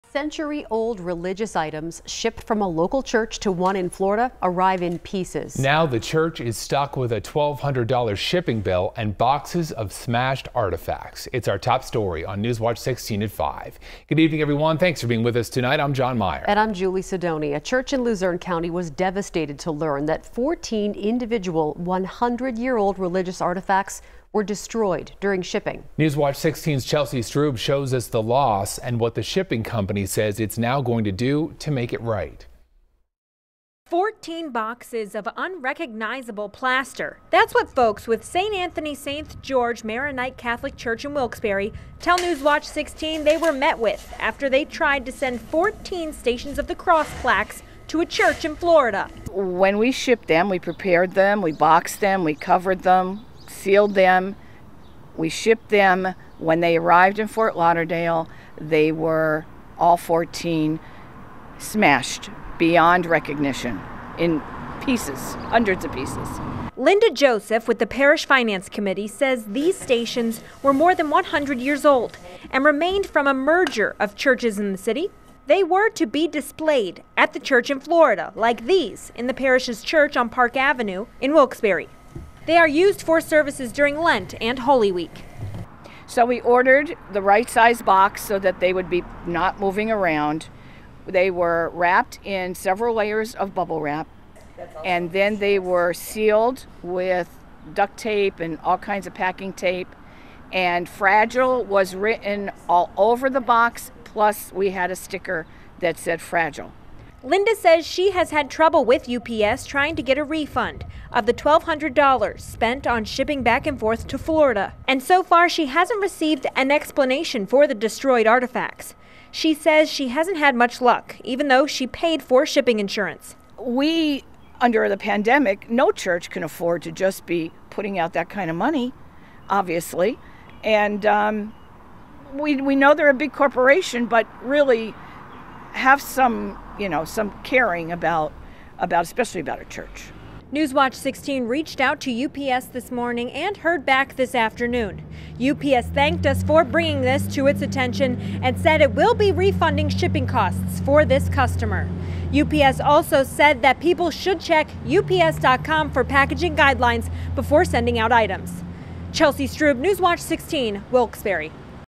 Stations of the Cross smashed during shipping
Newswatch 16's Chelsea Strub shows us the loss a parish is facing and what the shipping company says it's going to do to make it right.